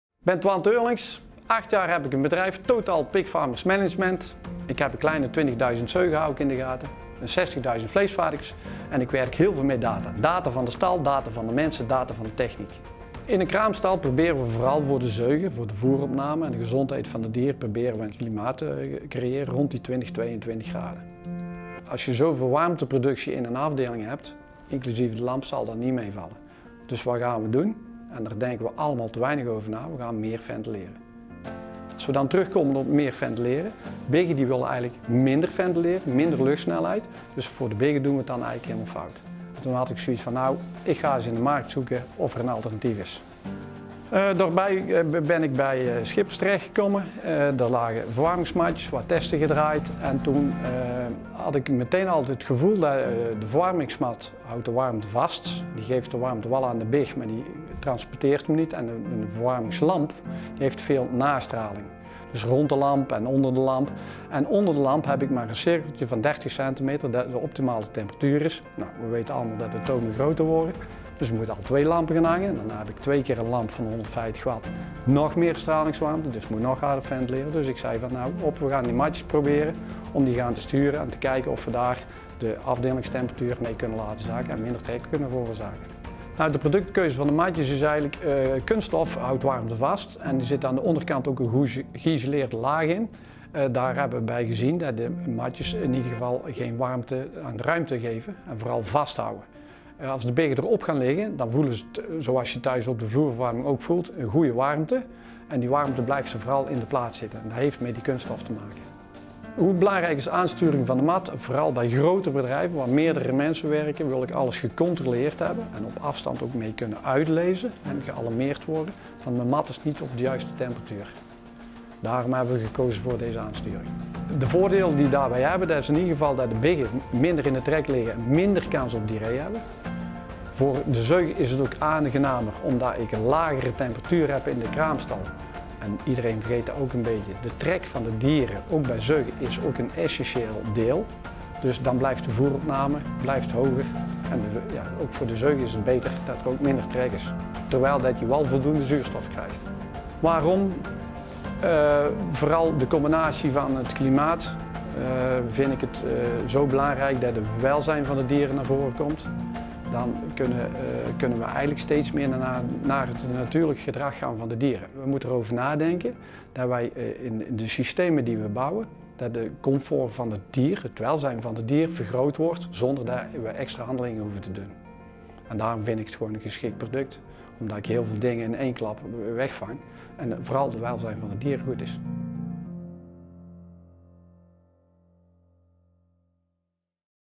Twan Teurlings over klimaat in de kraamstal | MS Piglet Optimal Comfort
Twan Teurlings is eigenaar van Total Pigfarmers Management.

In deze video legt adviseur Twan uit hoe belangrijk een optimaal klimaat in de kraamstal is en wat hij van de MS Piglet Optimal Comfort vindt.

Wil je meer informatie over de MS Piglet Optimal Comfort? Kijk dan op of neem contact op met je adviseur.